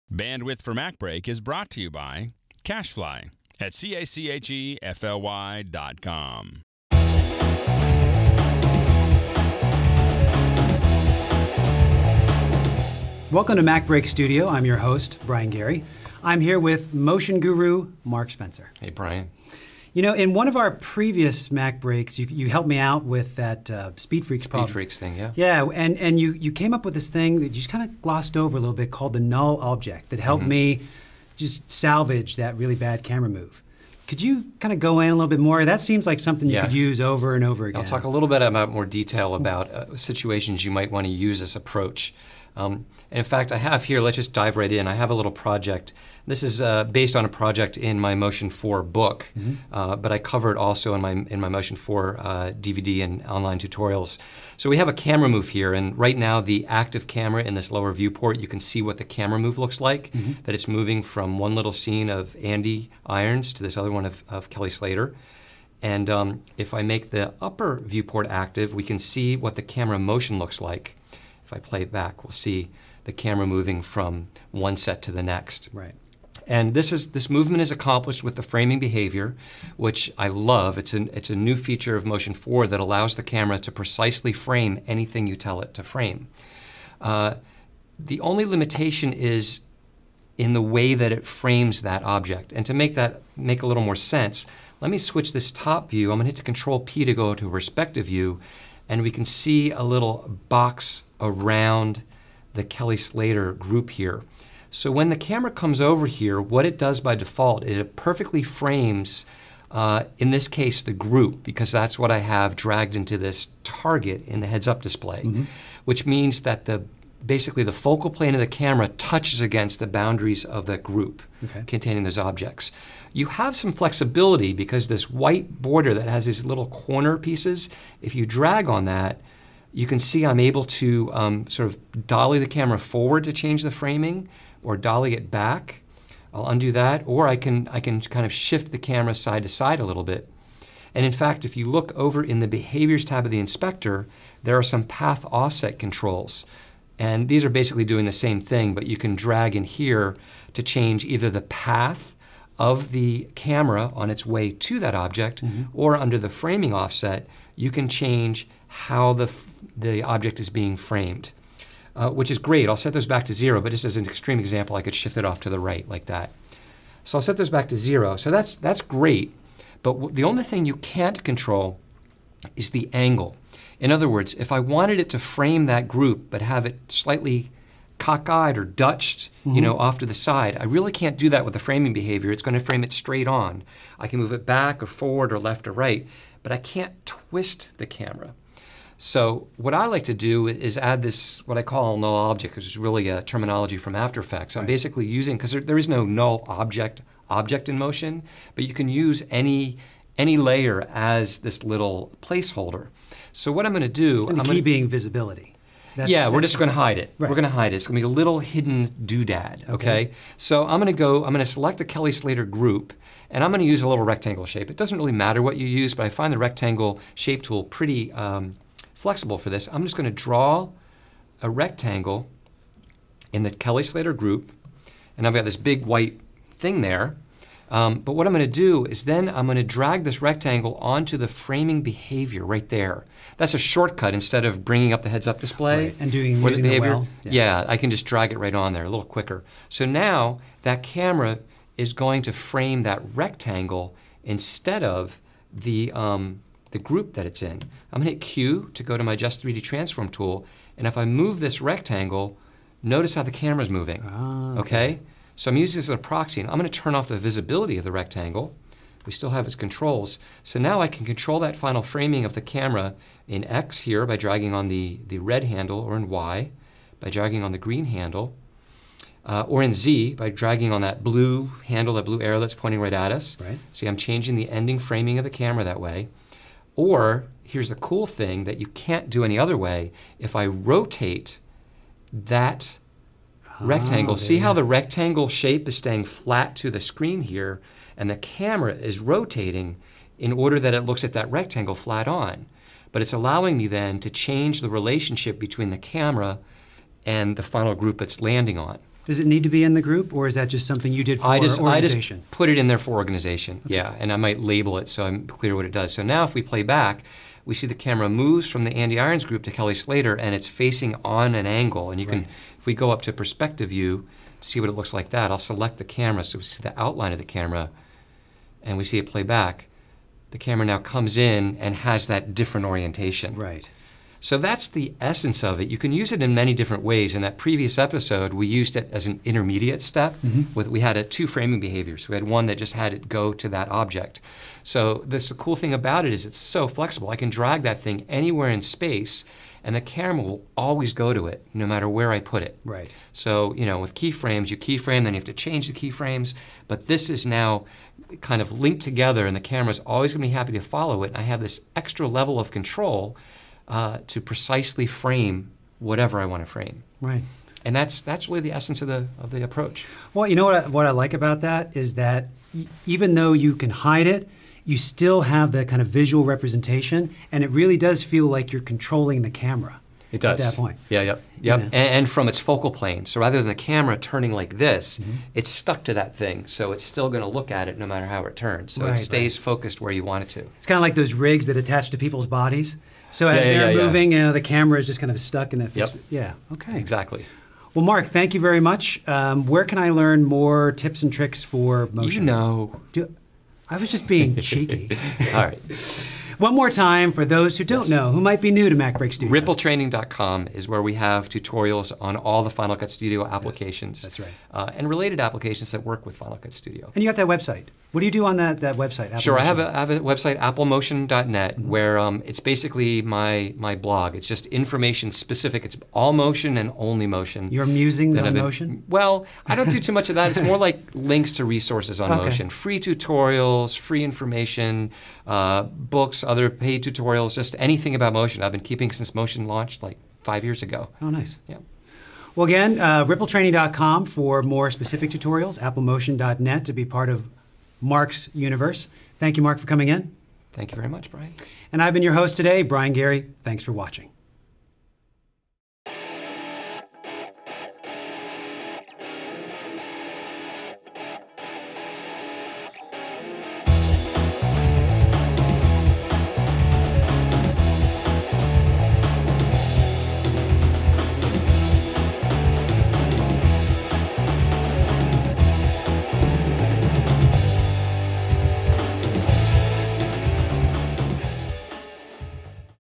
Using NULL Objects in Motion
Mark Spencer of Ripple Training shows Brian Gary when it's appropriate to use NULL objects in Motion.